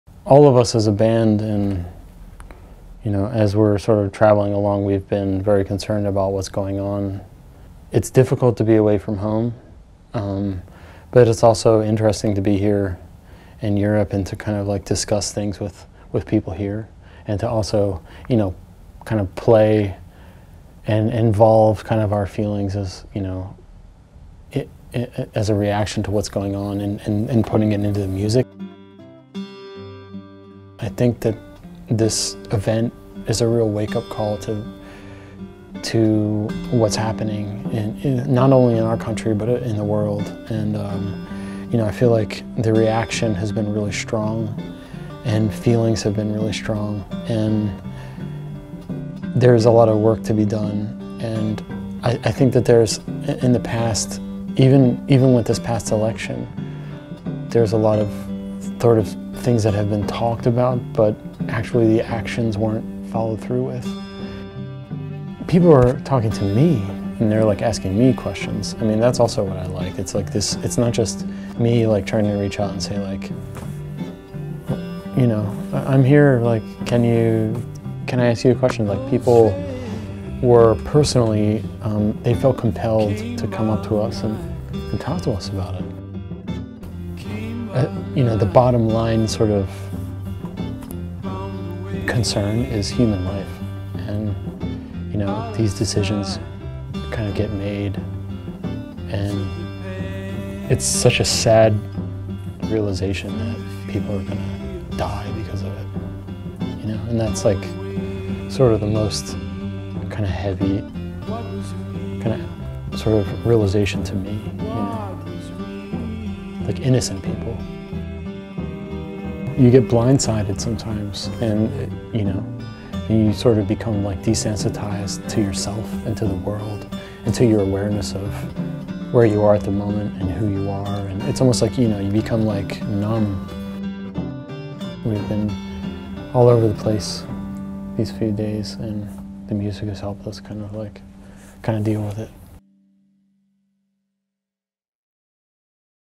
Welcome: Steve Gunn at Le Guess Who? 2016
Le Guess Who? & de Volkskrant bring you video portraits of artists performing at Le Guess Who? 2016.
We met up with Steve Gunn backstage, and talked about current political affairs, how it affects being on the road and performing music. "We've been all over the place these few days, and music has helped us deal with it." Afterwards, we captured his show at TivoliVredenburg's Pandora.

Host: Jasper Willems
Directed by Nick Helderman
Sound: Bart Hoevenaars